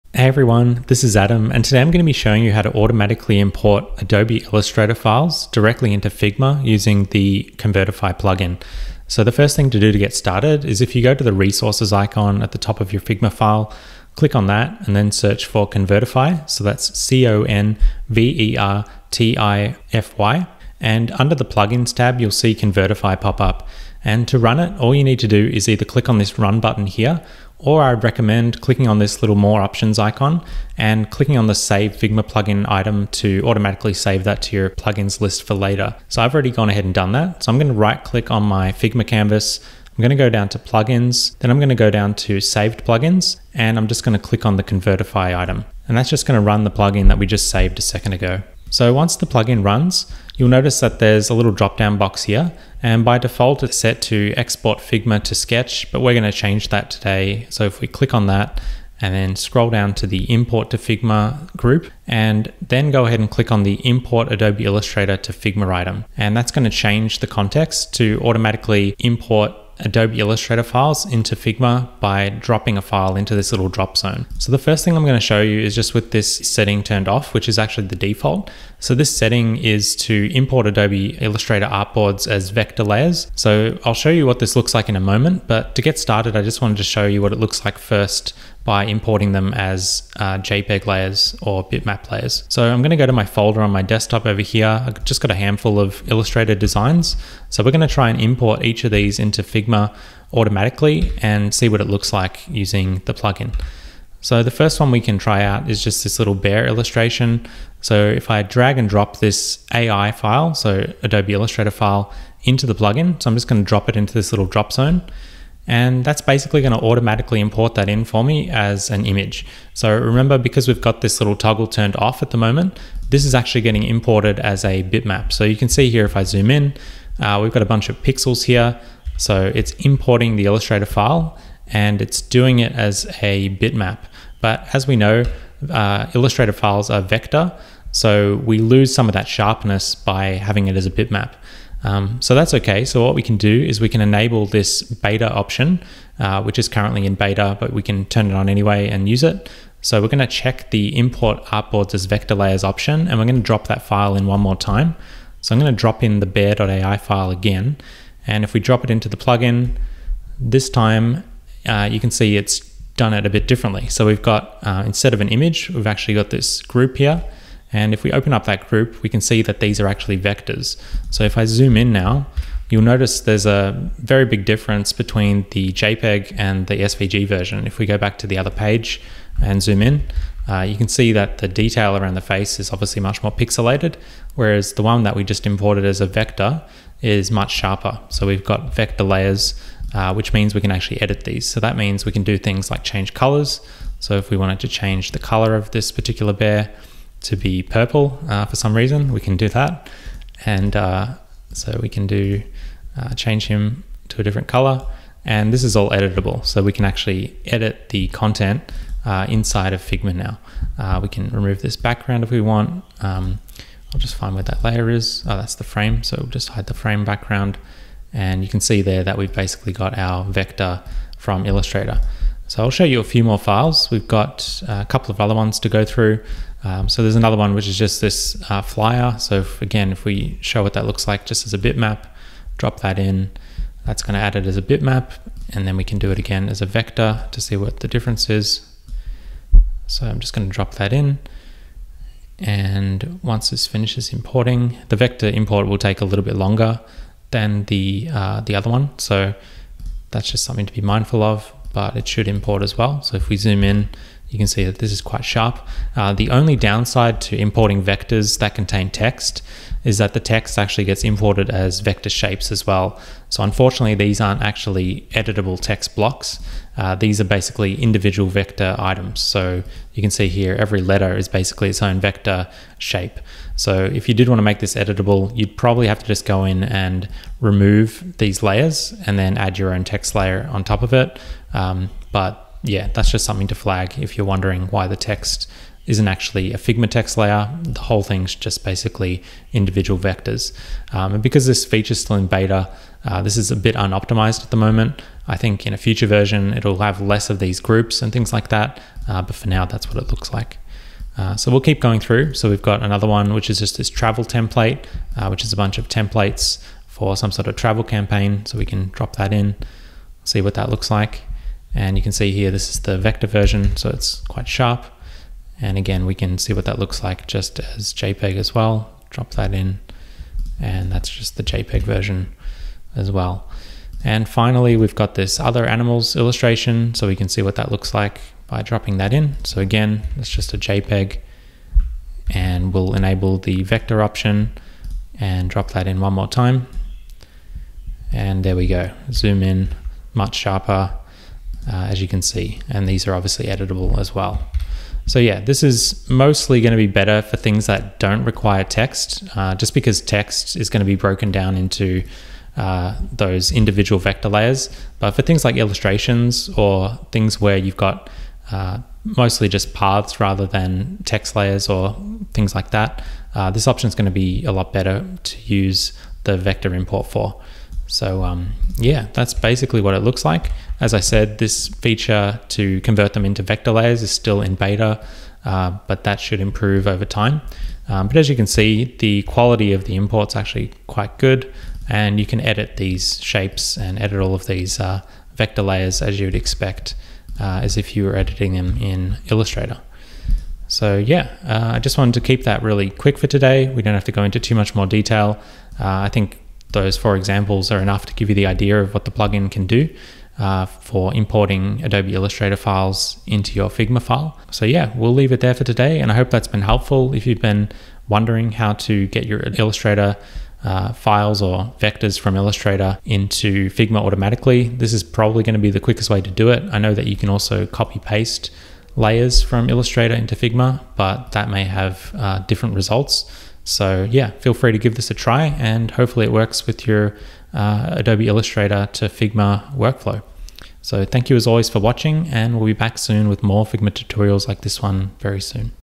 Figma Tutorial: Import Adobe Illustrator files to Figma with one click
This video tutorial is a complete step-by-step guide showing you how to automatically import your Adobe Illustrator (.ai) files to Figma using the Convertify plugin